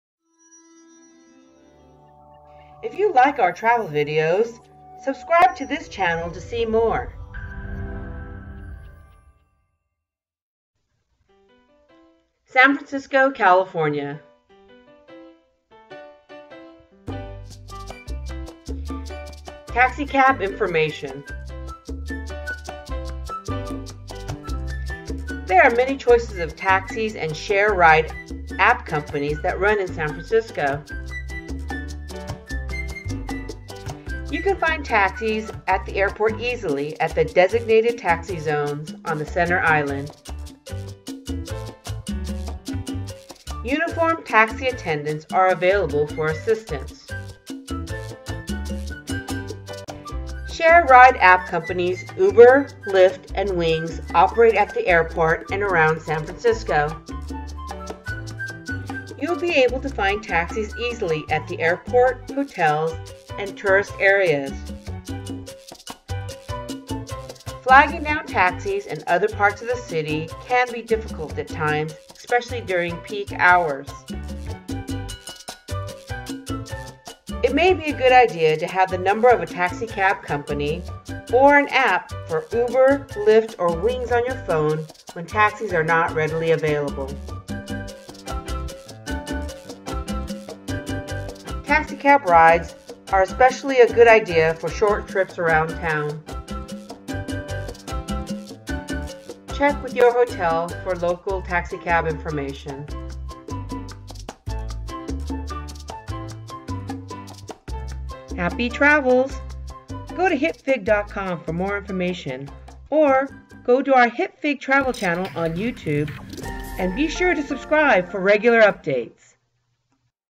San Francisco - Taxi Cabs Guide - Getting Around (TRAVEL GUIDE) | Episode# 3
San Francisco Taxi cabs helpful information in this Guide for new visitors in this San Francisco by Hipfig. San Francisco has currently 4800 taxi drivers and Taxis are readily available in the SF downtown area.

Topics covered in this useful San Francisco Taxis travel guide for visitors are:
1). San Francisco Taxi cabs information,
2).  How to catch a Taxicab in San Francisco?
3).  Tips for getting on to Taxi Cabs in San Francisco,
4). Travelers Tips on riding Taxi Cabs in San Francisco, and
5). Ride-share Apps Cars like Uber & Lyft in San Francisco.